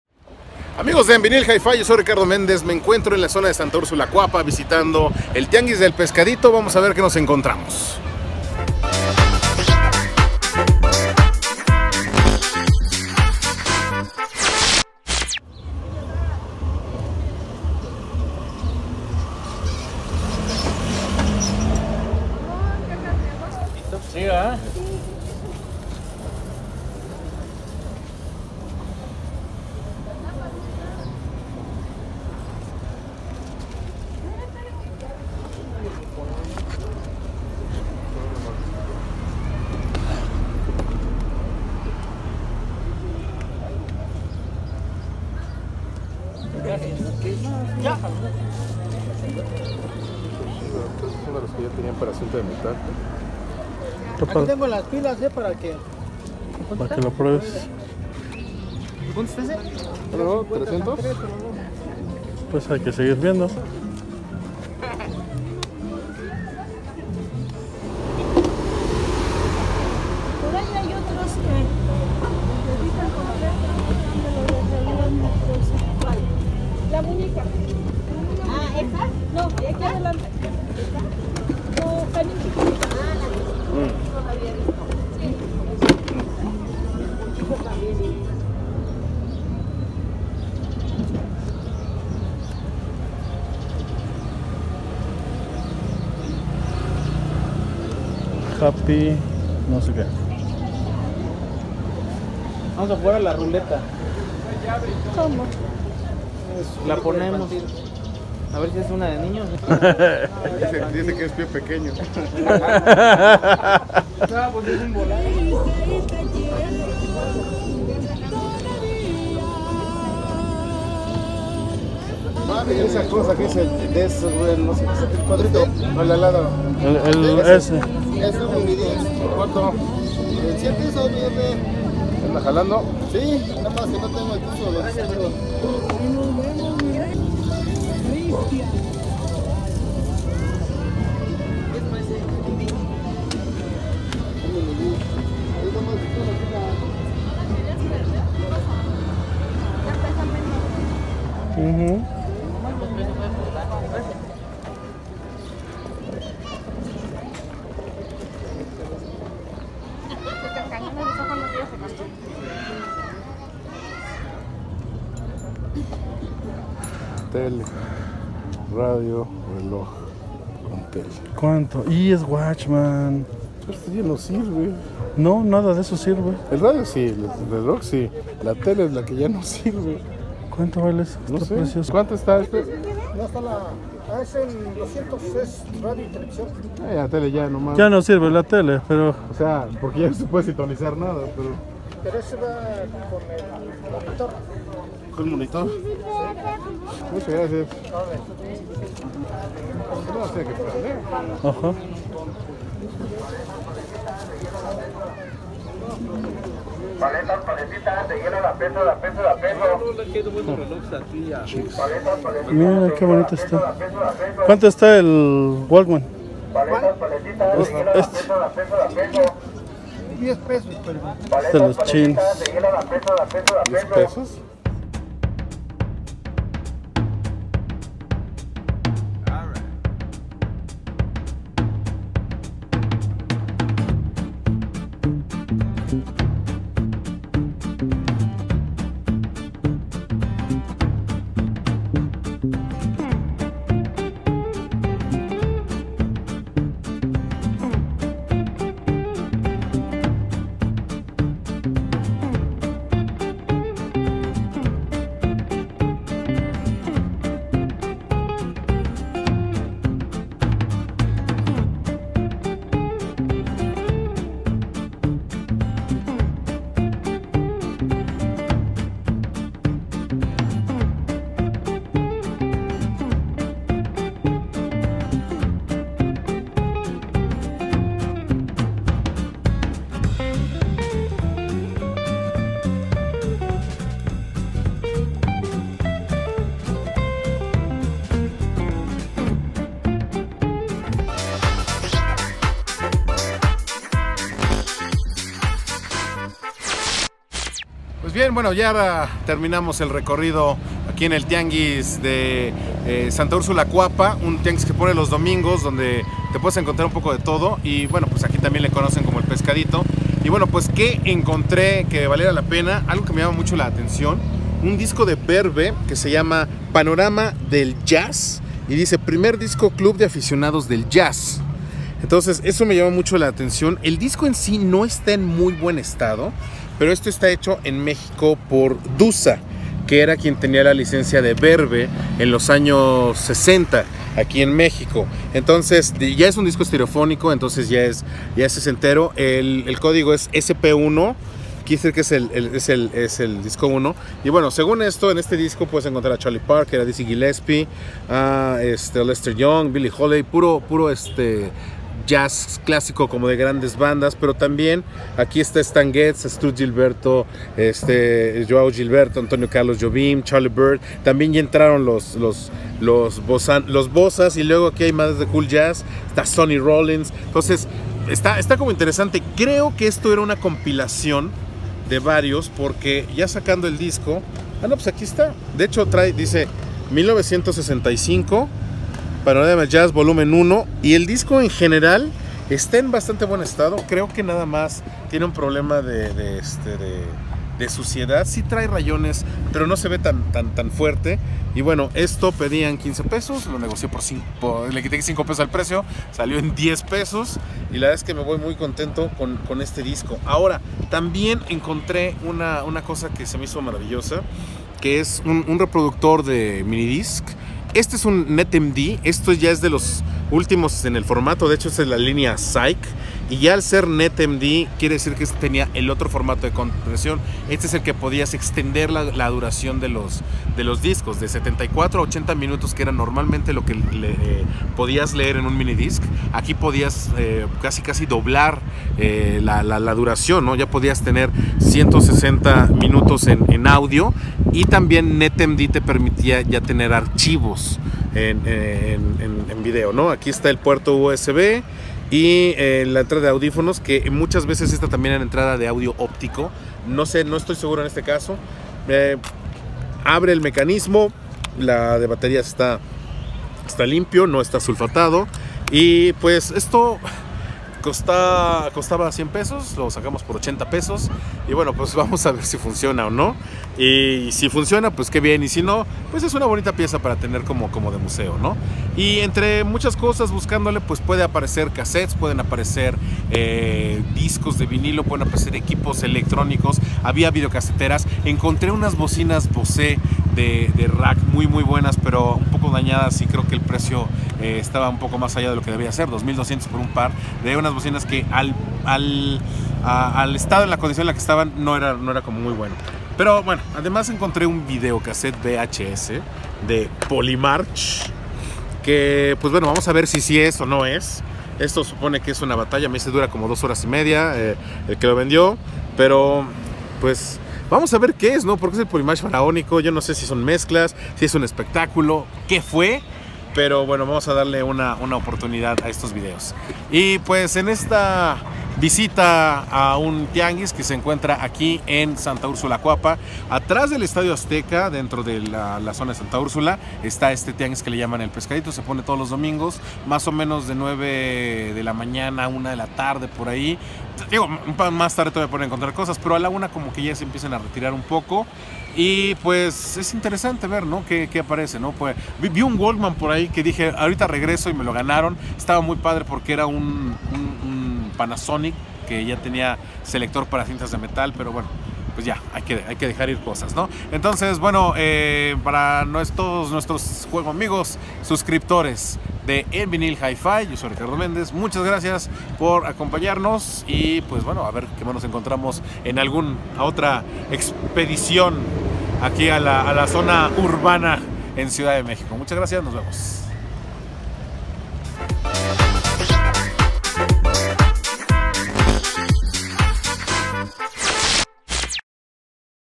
En VINYL Hifi - Chachareando en Tianguis El Pescadito
buscando equipo y discos haber que sale en este